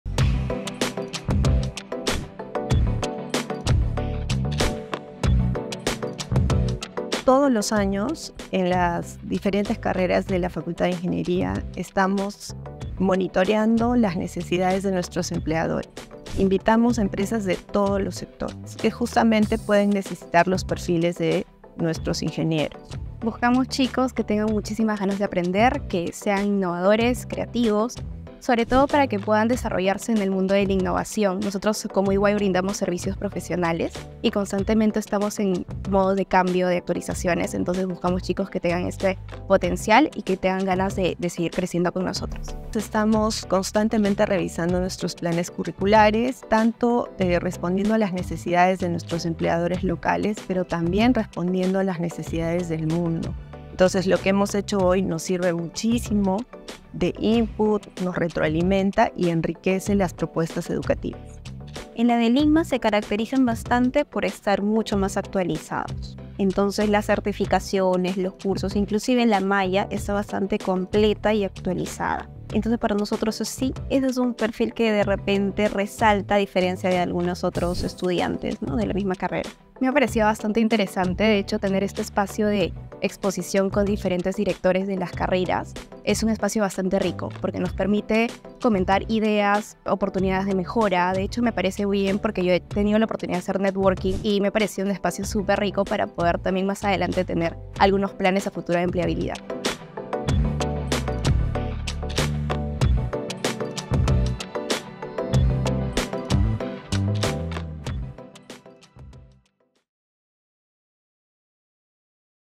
ULIMA - World Café 2024
Analistas de selección, psicólogos y asistentes de atracción del talento y recursos humanos de reconocidas empresas como BDO Perú, Cosapi, Entel Perú, Ferreycorp, Interbank y Prima AFP, entre otras, participaron del World Café 2024, un desayuno de trabajo organizado por la Facultad de Ingeniería de la Universidad de Lima, con el objetivo de presentar la malla curricular de las tres carreras que la conforman y conocer lo que requiere el mundo laboral en estos campos gracias al 'feedback' entre academia y empresa.